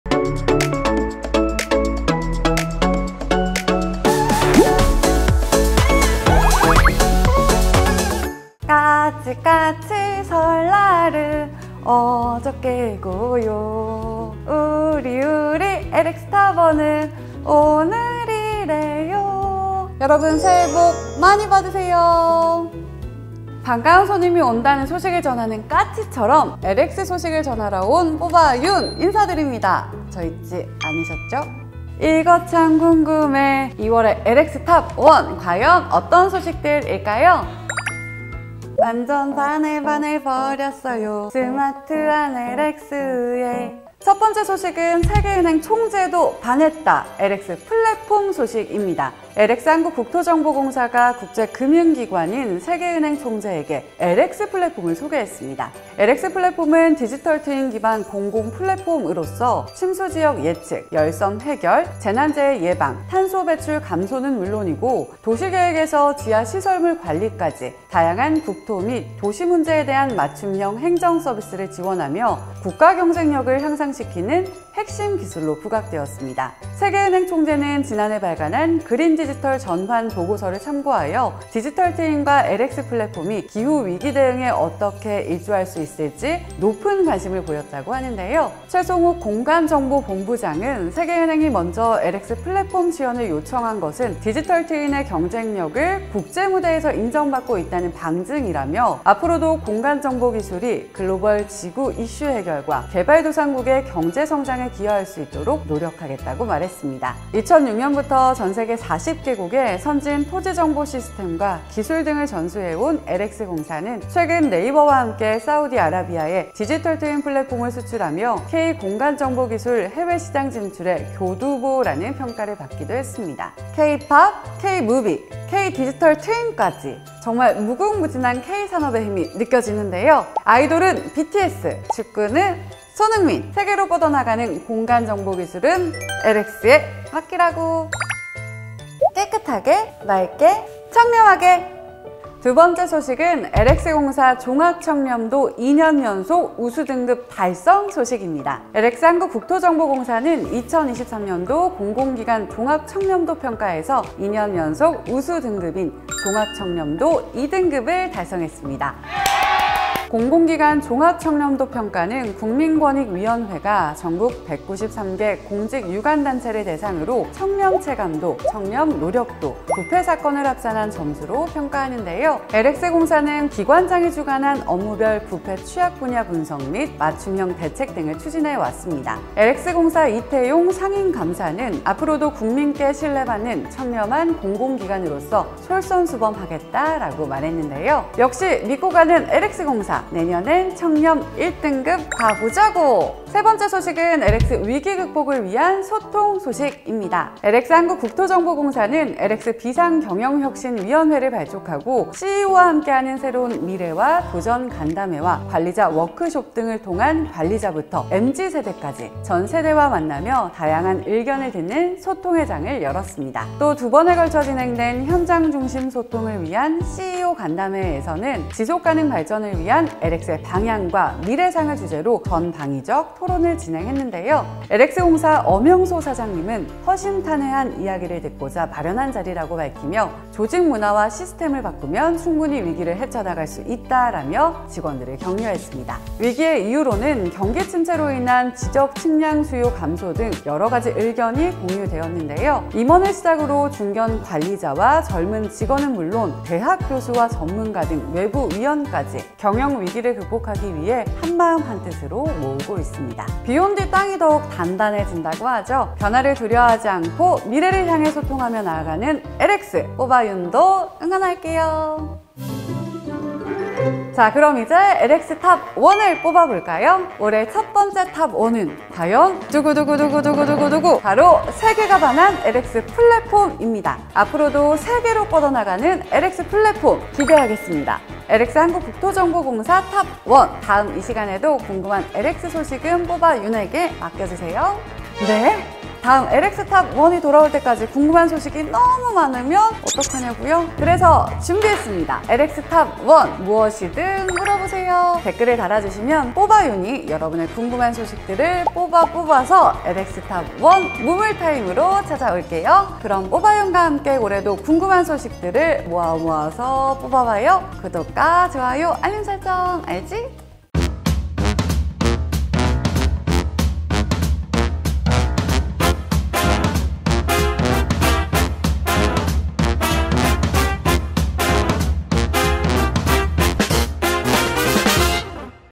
BTS, 손흥민, LX플랫폼 Let’s go!🇰🇷 K-산업을 선도하는 LX의 놀라운 기술력🫢 [LX TOP1 2월 호]
LX의 새로운 소식을 전하는
LXTV의 마스코트, 뽑아윤이 돌아왔어요💚

청룡의 기운을 가~득 받아
2월 호엔 더욱 기운 넘치는 소식들이 있는데요!

애국심 뿜뿜 차오르는 뉴스부터,
종합청렴도 우수등급 달성,
LX 위기극복을 위한 소통의 장 개최 소식까지!👏

2024년에도 LX는 더 나은 미래를
만들기 위해 열심히 달려볼게요!!🫡

⏰LX TOP1 2월 호 타임스탬프⏰
[00:00] 2월 호 시작!
[00:39] 세계은행 총재도 반했다! LX플랫폼
[02:19] LX 공사, 종합청렴도 2년 연속 우수등급 달성
[03:14] LX 위기극복을 위한 소통
[04:24] 2월 호의 LX TOP1은 과연?!